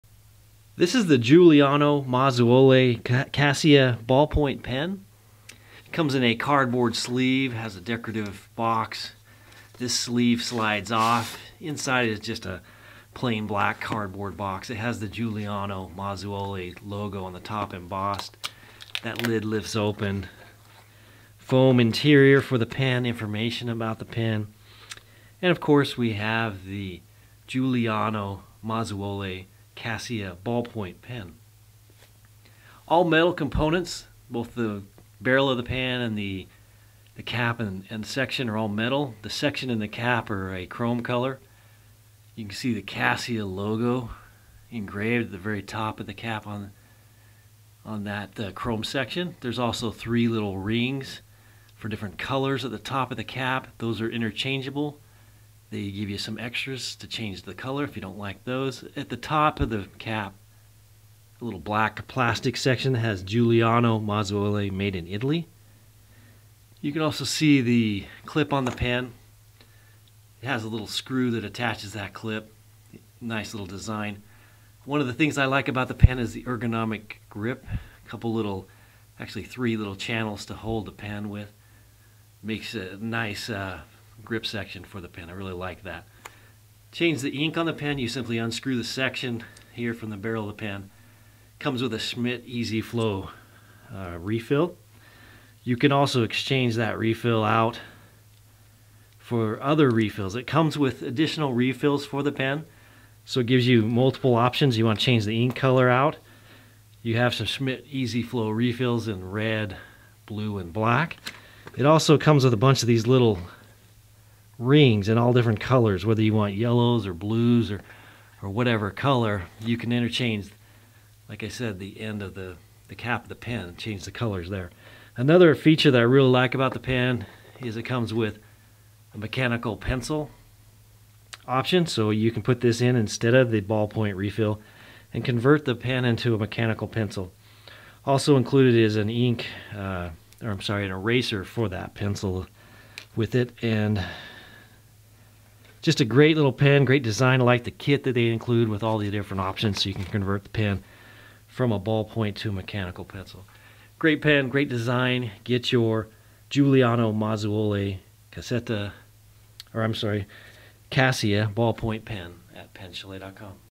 Giuliano Mazzuoli Cassia Ballpoint Pen
Details about the Giuliano Mazzuoli Cassia Ballpoint Pen

See the Giuliano Mazzuoli Cassia Ballpoint Pen up close. Learn how to change the ink. See what comes with the pen and how it is packaged. Get your Giuliano Mazzuoli Cassia Ballpoint Pen